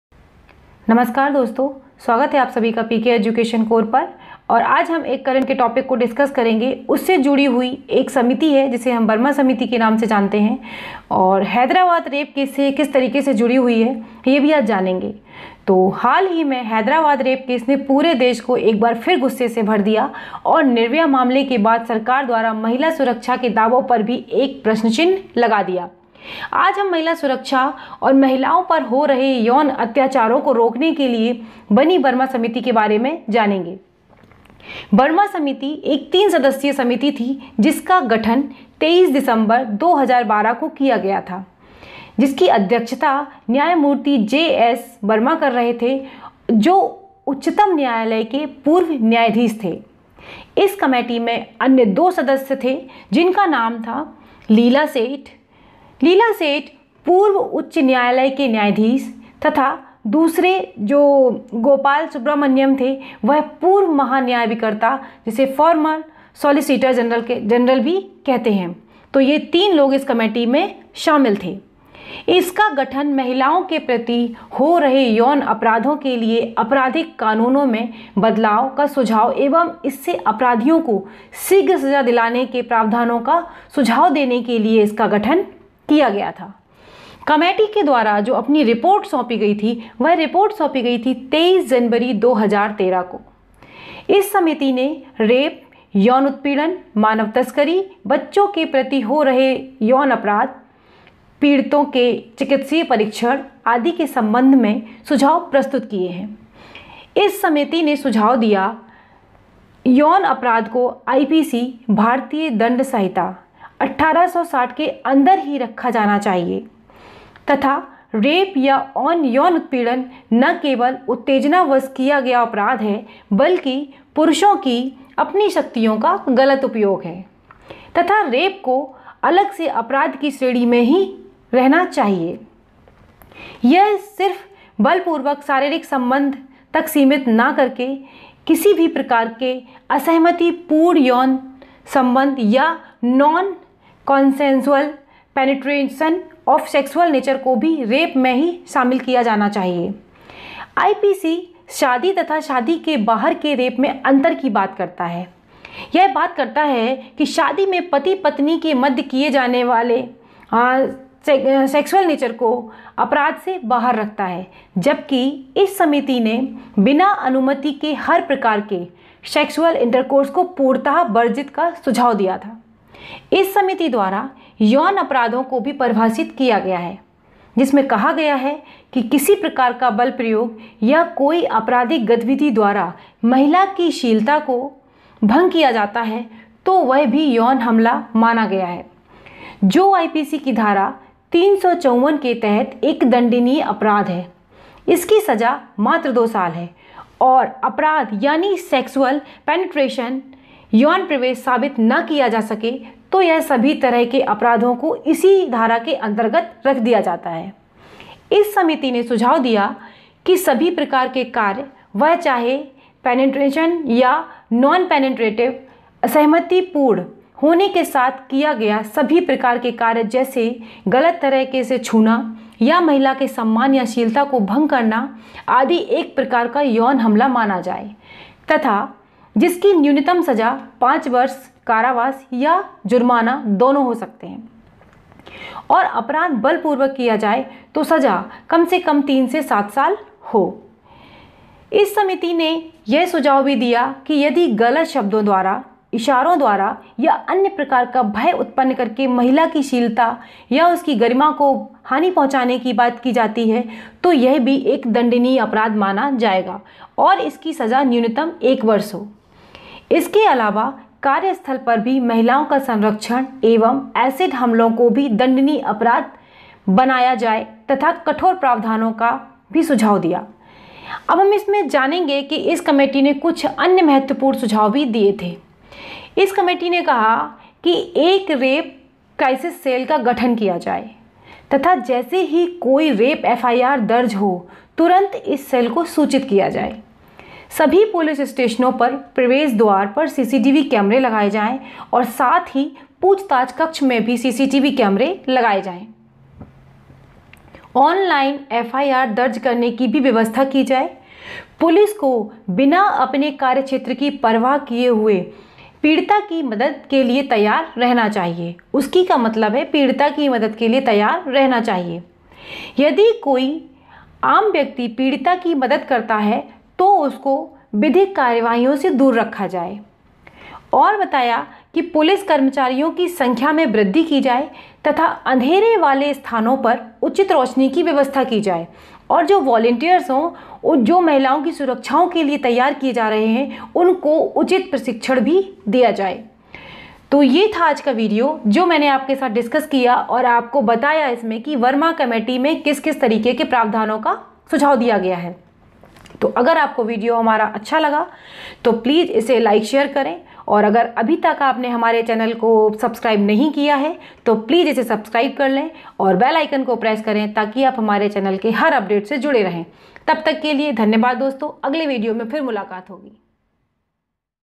pkeducationcore वर्मा समिति , Uppsc, upsc ,mppsc, Current topic
Hello friends,  in this video i will tell you about Justice Verma Committee . This video is in Hindi. It is one of the most important topics for all exams like UPPSC, UPSC. I have given my best to capture the most of the Current Affairs. I hope you will like it and may be benefited.